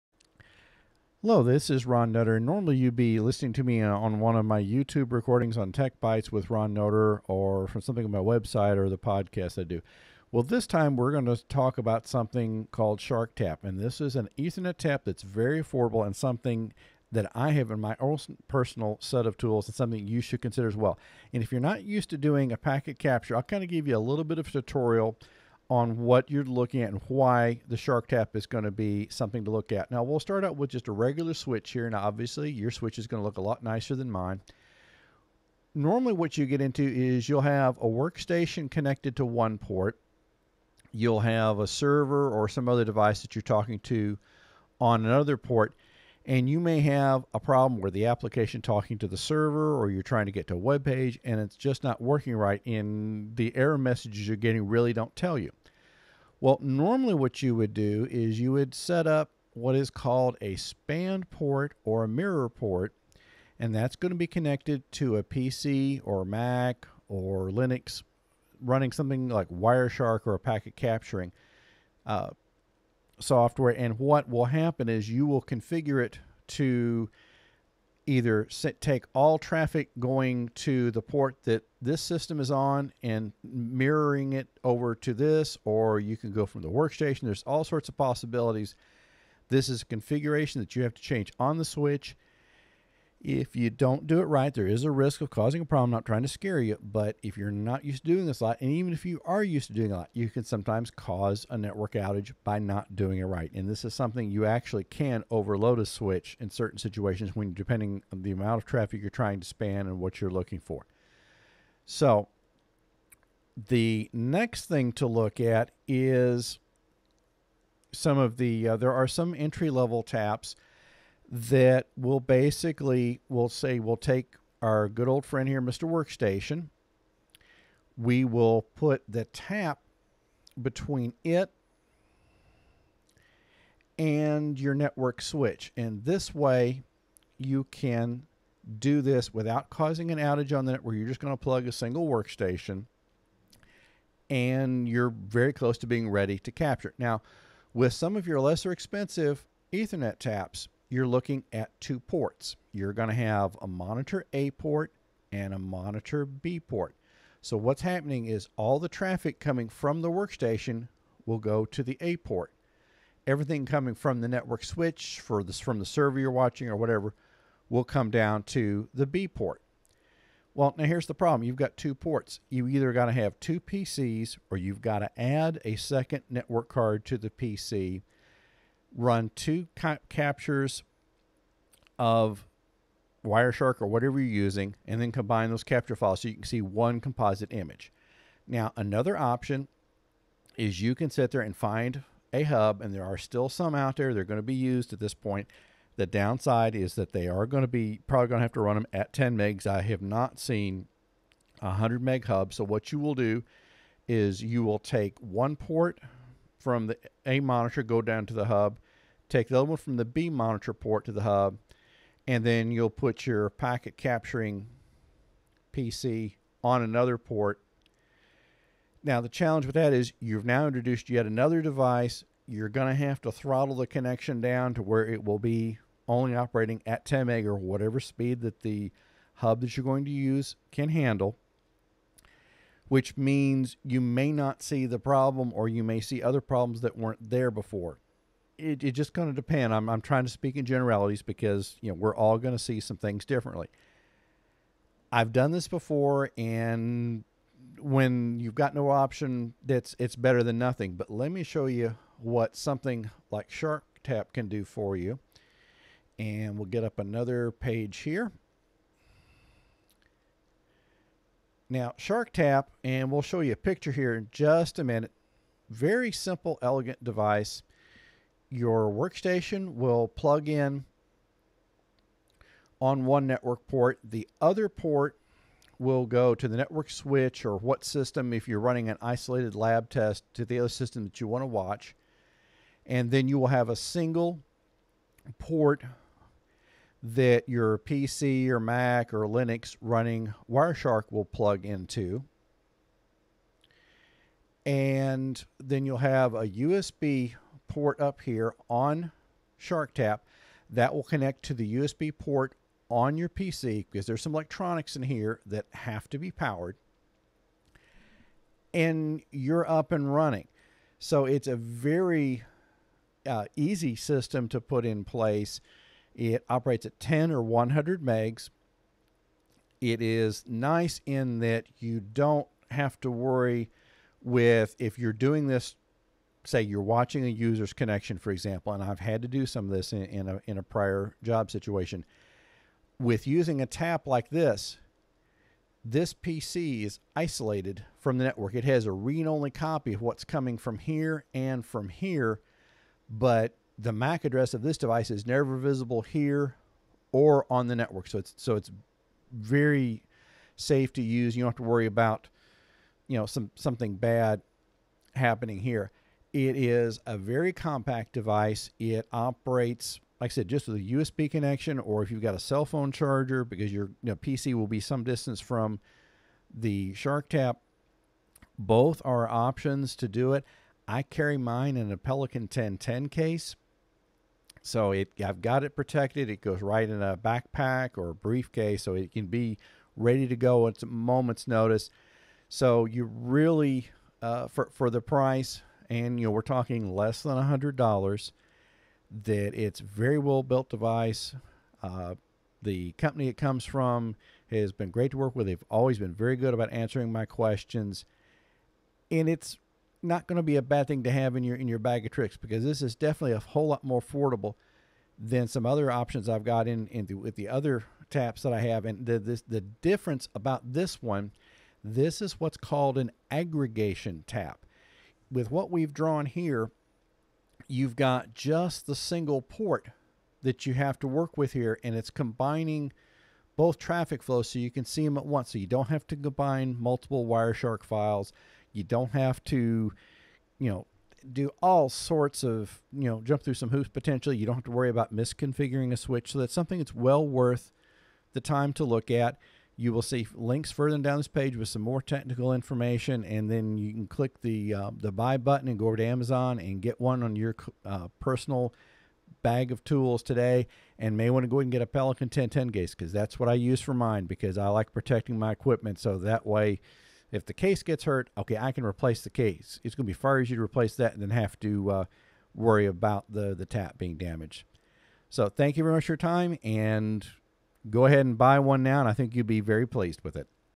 SharkTap Ethernet Tap Demo Video
I recently became aware of a ethernet tap that is affordable and versatile.  It is known as the SharpTap.  This version is the 2nd generation of what was initially brought to the attention of the Raspberry Pi community.  In this latest version, it is in a more ruggedized package that increases its versatility and usefulness.  I carry my person SharpTap in a Pelican 1010 case which is very affordable and does a great job at protecting your SharpTap from damage.